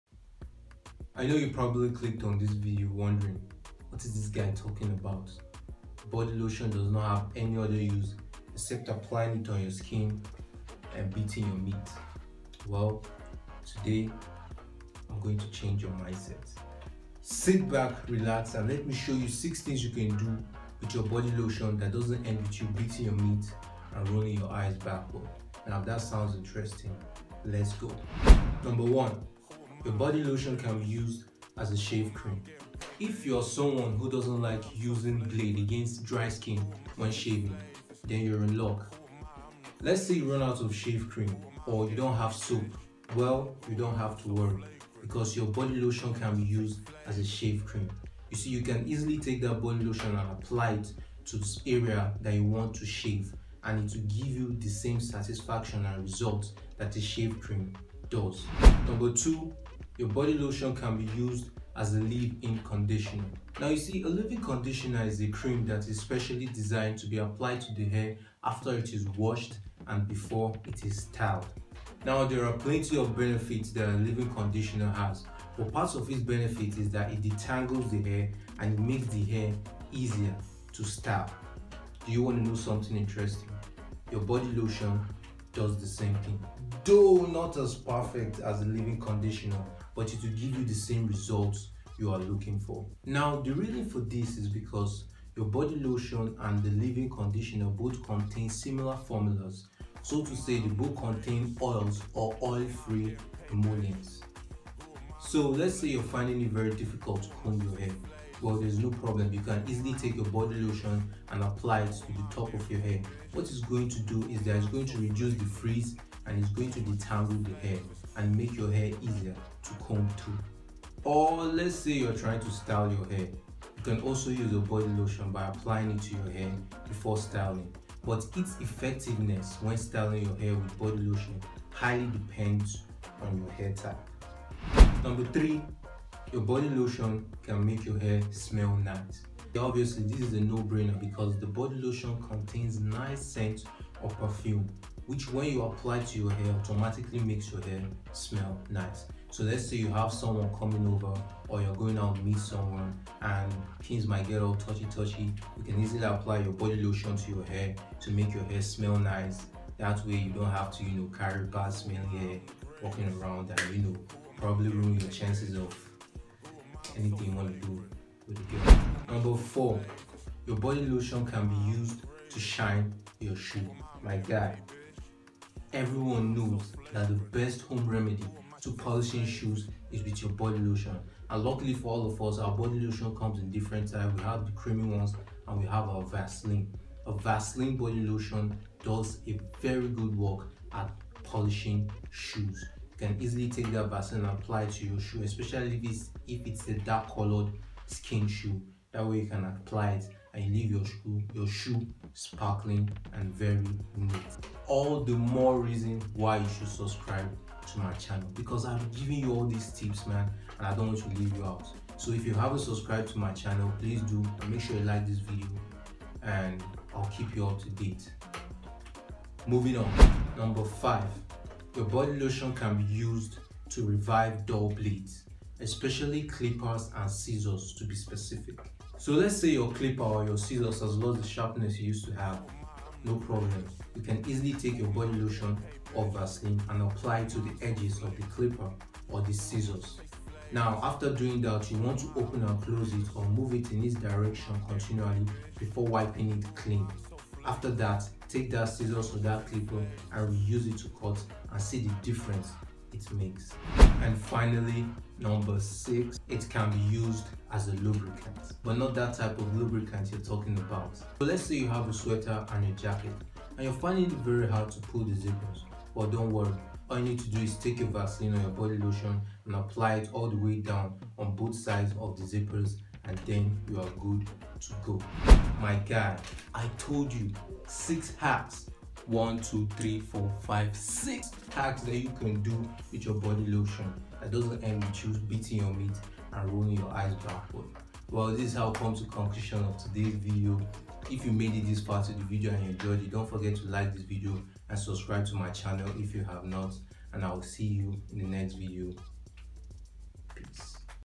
6 Odd But Useful BODY LOTION Life Hacks
most people believe the only service  BODY LOTION renders  is it's application to our skin, and maybe touching ourselves with it( for those who are into it ).
But there are a variety of ways our body lotion can be of great help to us and these ways (hacks) can be implemented in our day to day life. So sit back, relax and let me show you how..